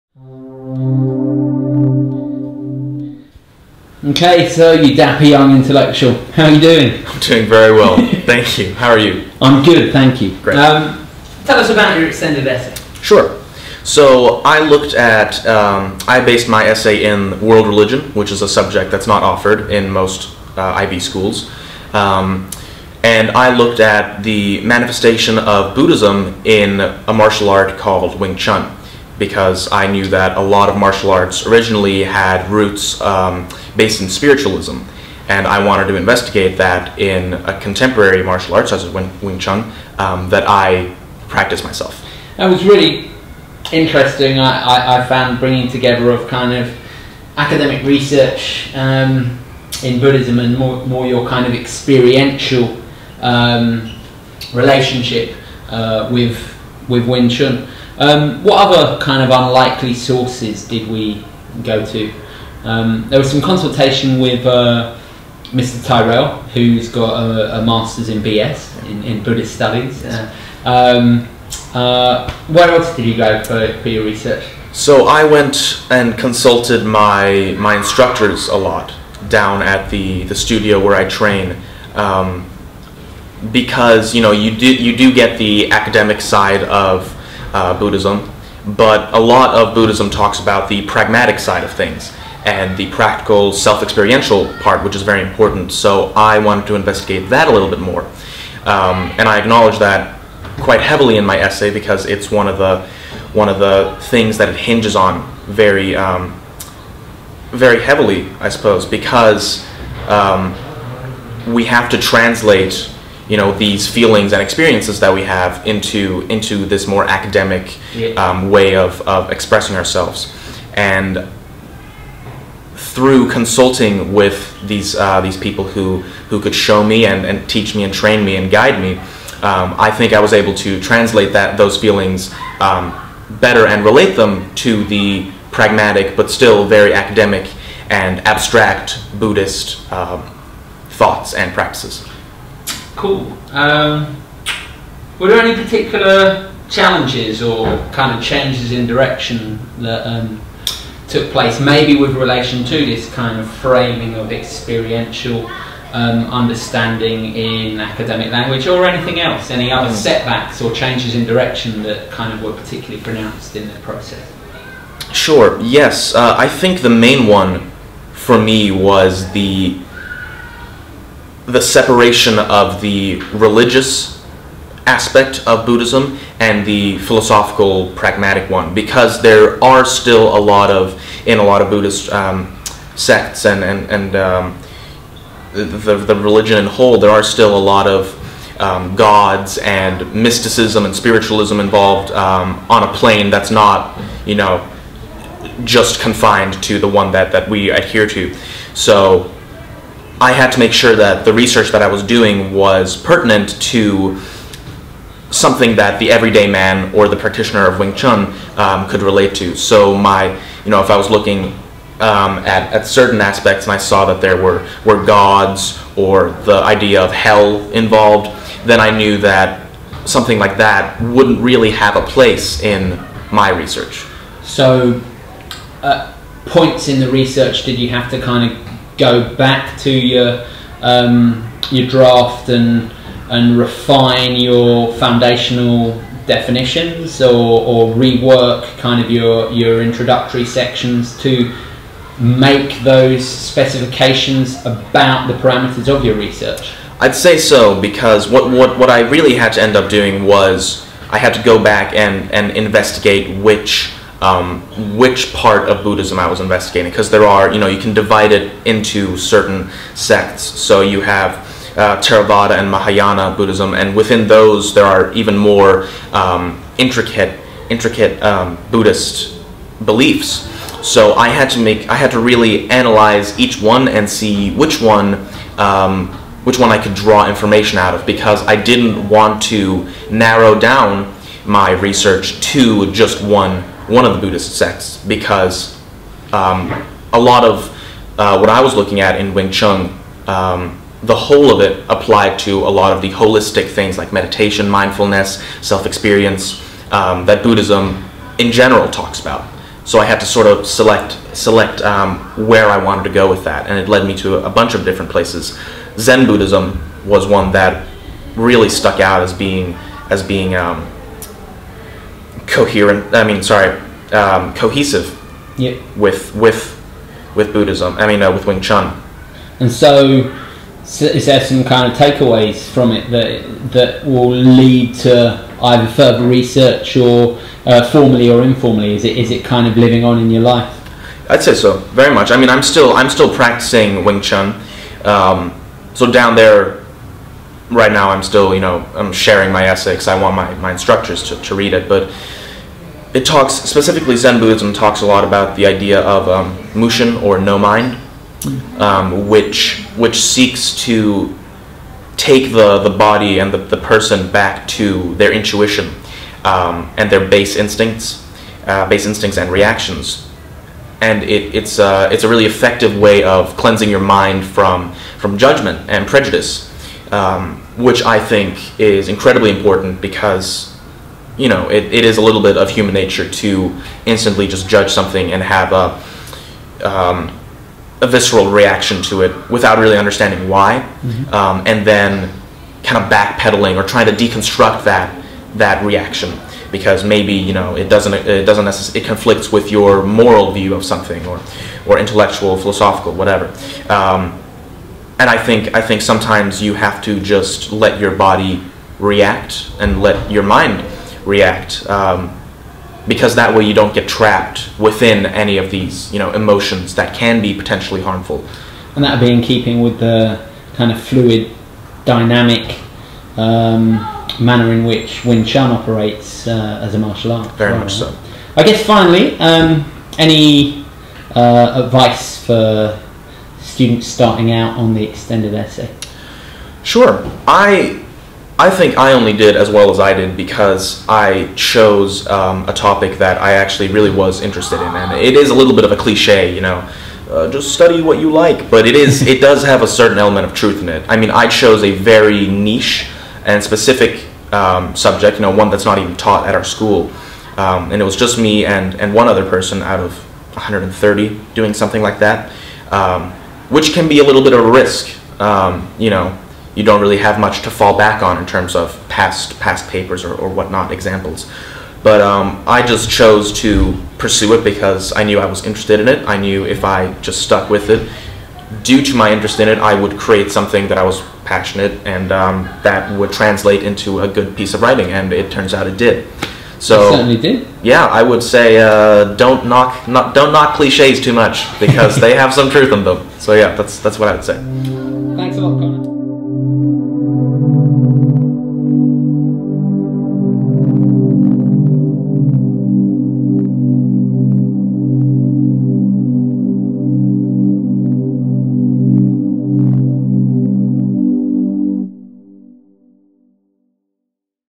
Conor's Extended Essay: Buddhism Manifest in Wing Chun Form
How have the practices and teachings of Buddhism manifested themselves into the martial art of Wing Chun?

The Con-man talks through his process, experiential vs academic research, and intrinsic motivation.